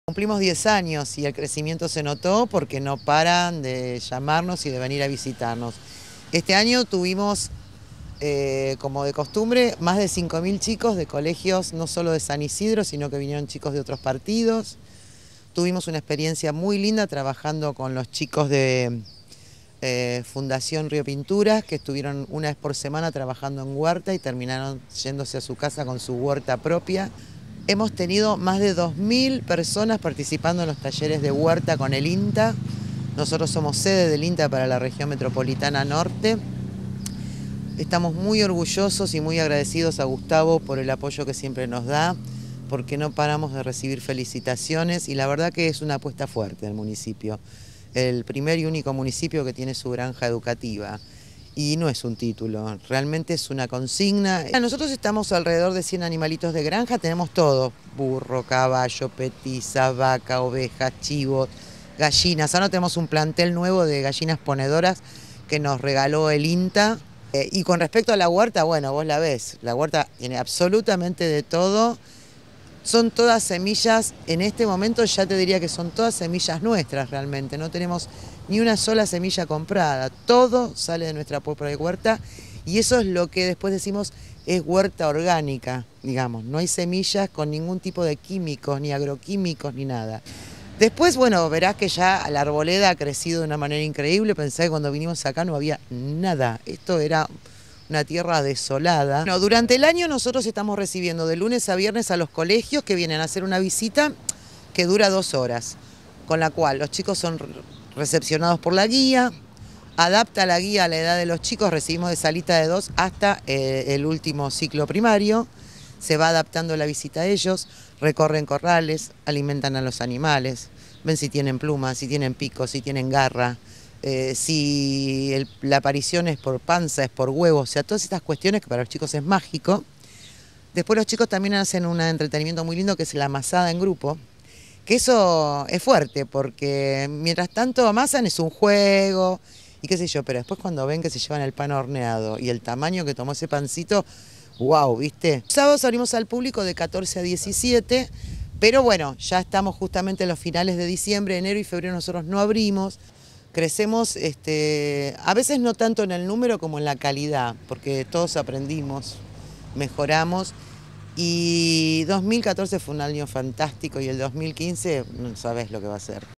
En 2014, más de cinco mil chicos visitaron la Chacra San Isidro Labrador
El predio que recrea el ámbito rural está ubicado en Villa Adelina, tiene más de 100 animales y una huerta orgánica. De marzo a diciembre, realiza visitas guiadas y los sábados abre al público en general.

Durante 2014, más de cinco mil chicos, disfrutaron de las propuestas naturales que ofrece la Chacra San Isidro Labrador (Av. Perito Moreno 2610, Villa Adelina). Los principales visitantes fueron alumnos de jardines de infantes y escuelas de todo el distrito, y jóvenes que asisten a instituciones para personas con discapacidad.

Además, como el predio es sede del Instituto Nacional de Tecnología Agropecuaria (INTA), dos mil personas participaron de los talleres de la huerta orgánica que funciona en el lugar.

“Buscamos aportar nuestro granito de arena para que los sanisidrenses se conecten con la naturaleza y respeten el medio ambiente. Trabajamos con mucho compromiso y cariño. Año a año crecemos en número y calidad”, afirmó Mabel Leva, directora de Educación Ambiental de San Isidro.

!cid_DD10660B25F24A4C858024FF647390E5@dhipolito

La Chacra San Isidro Labrador cuenta con alrededor de 100 animales (burros, caballos, vacas, ovejas, chivos, gallinas, conejos, patos y tortugas, entre otros) y la huerta en donde sólo se utilizan semillas propias, sin agregados químicos.

“Hay que empezar a entender lo importante de comer sano. El hecho de que los visitantes se lleven sus plantines y generen su propia huerta, en su hogar, sentimos que es un corazón verde ganado”, explicó Leva.

También, el predio de una hectárea ya tiene 86 árboles propios que se plantaron en distintos períodos.

Durante el año la chacra abrió sus puertas, de lunes a viernes, a los alumnos y los sábados, de 14 a 17, para el público en general.

“Las visitas consisten en recorrer los corrales, alimentar a los animales y hacer una amasada en grupo para luego llevarse el pan horneado a la casa”, concluyó Leva.

Durante enero y febrero, las actividades en la granja se encuentran suspendidas por las altas temperaturas. Se reanudarán a principios de marzo.